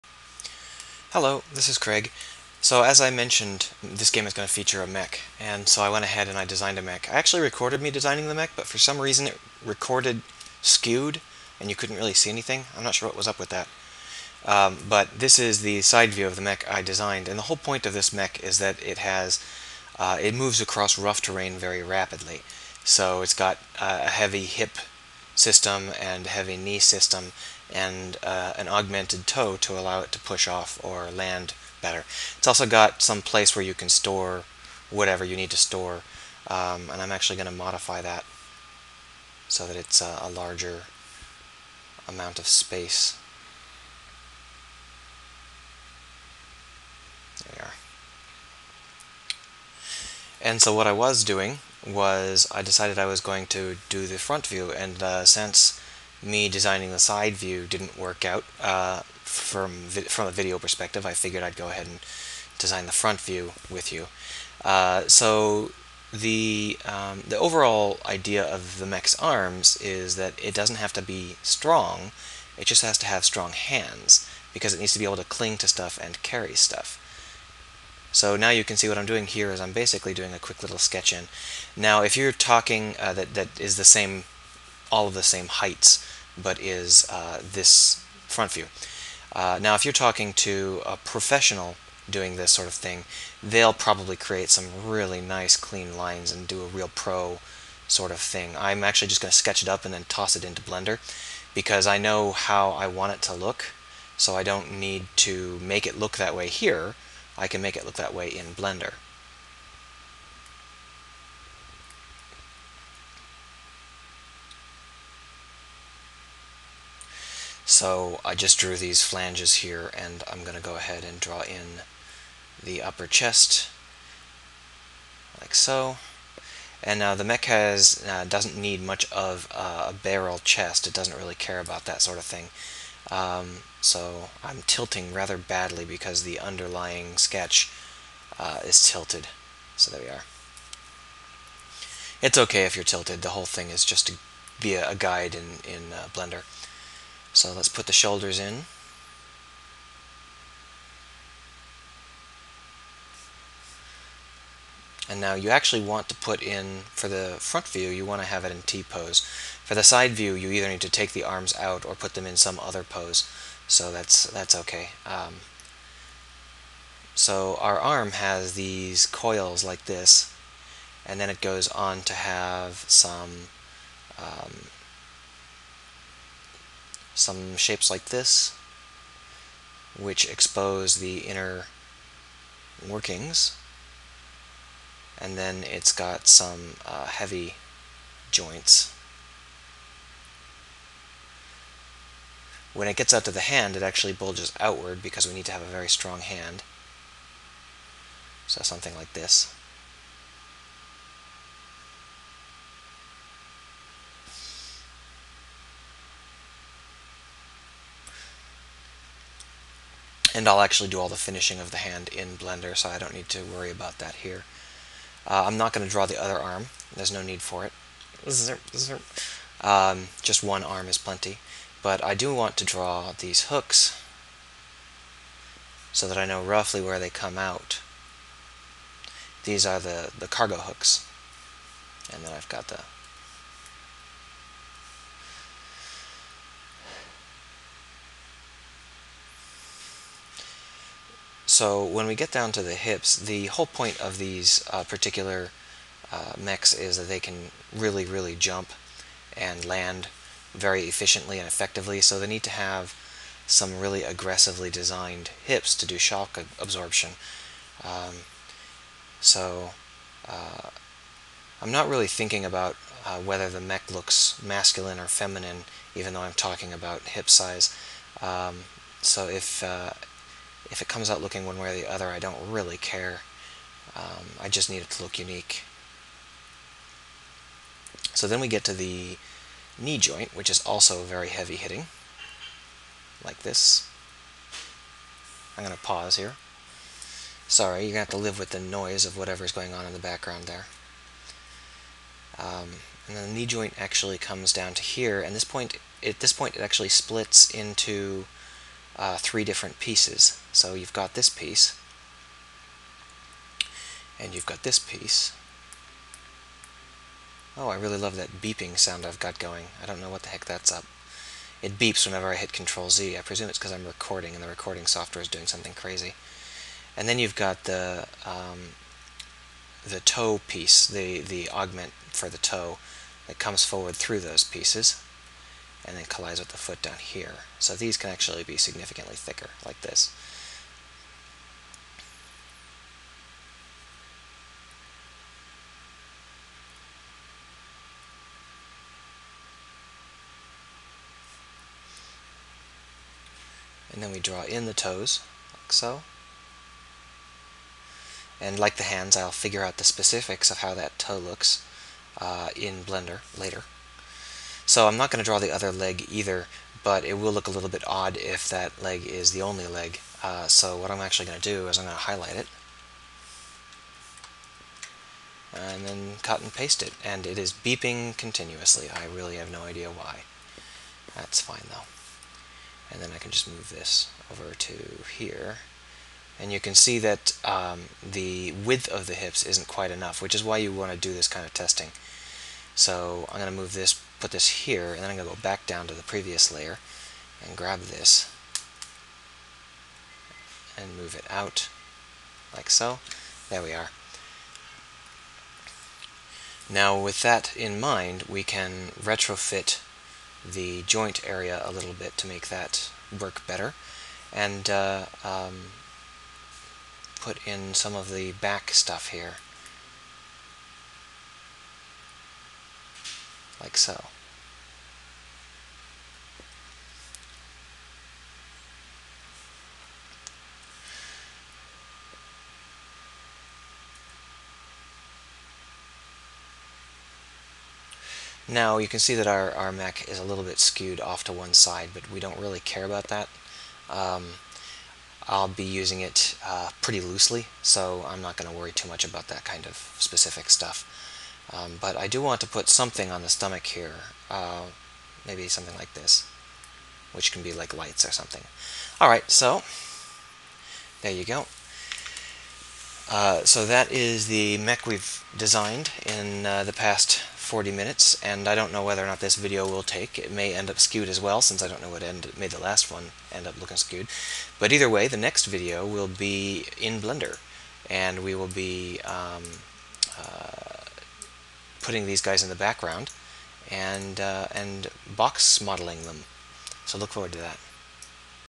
Minecraft Mech #20: Let's Design!
Sketchi sketchi.

I know absolutely nobody cares about this part, but I need to define the unique elements of my game so that I can start putting play elements in.

You shouldn't just build a world without understanding how you want it to play. The way physics and terrain works depend on how you want to play.